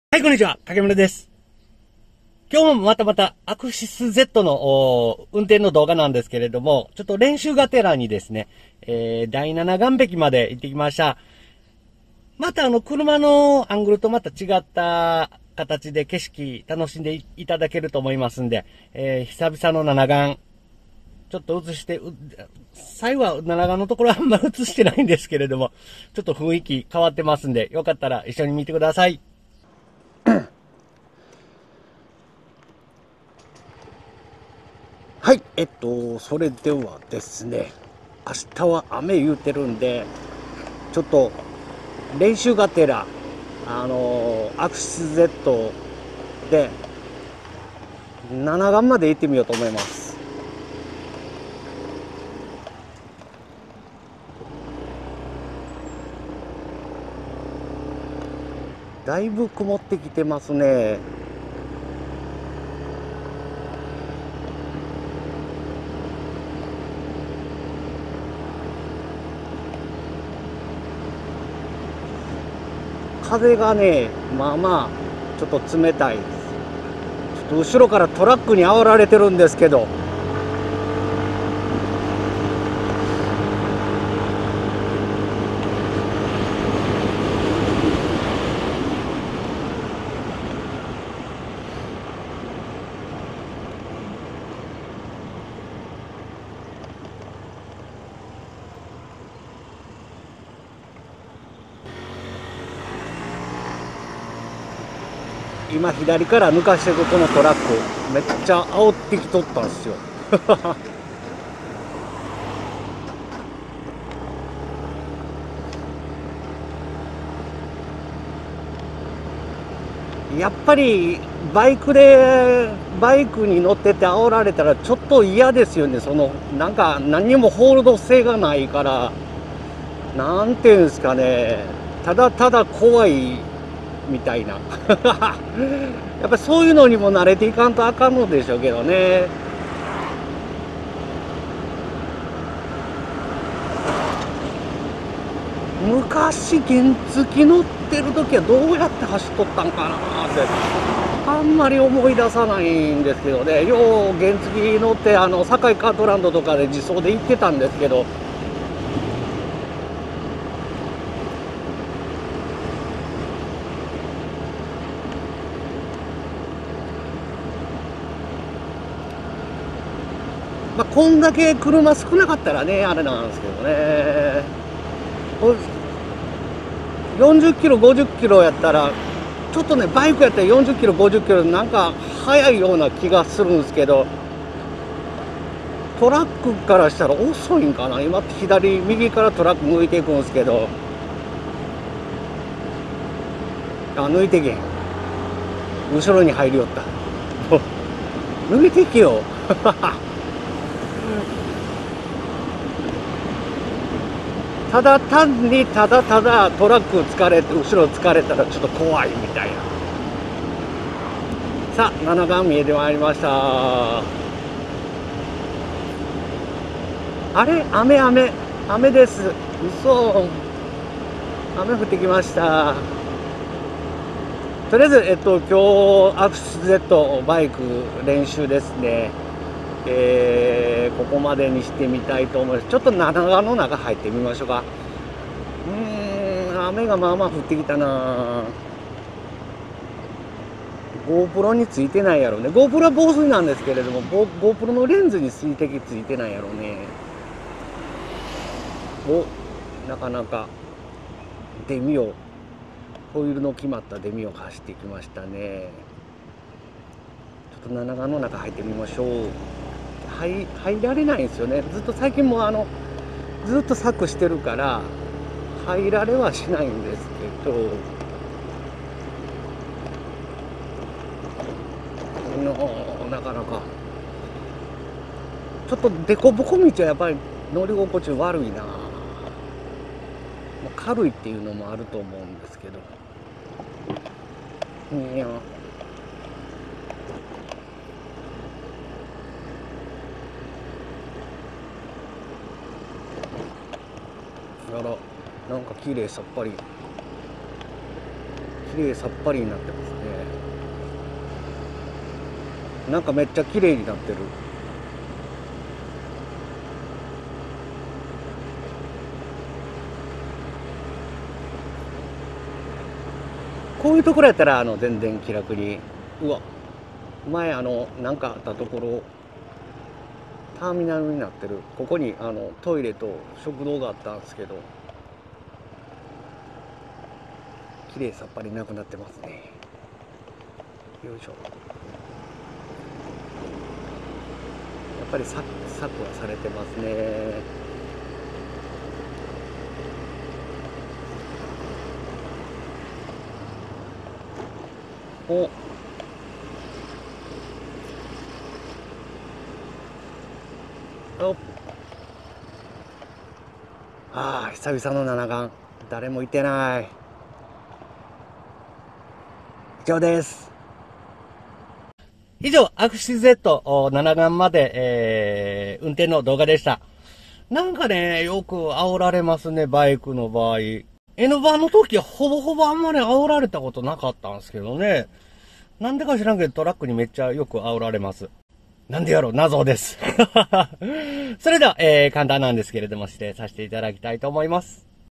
アクシスZを運転してナナガンまで行ってきました
単車に慣れるために、トラックの通行が多い道路を通って第七岸壁までアクシスZを運転する動画です。

撮影してほしい動画リクエストはこちらのコメント欄にお願い致します。m(_ _)m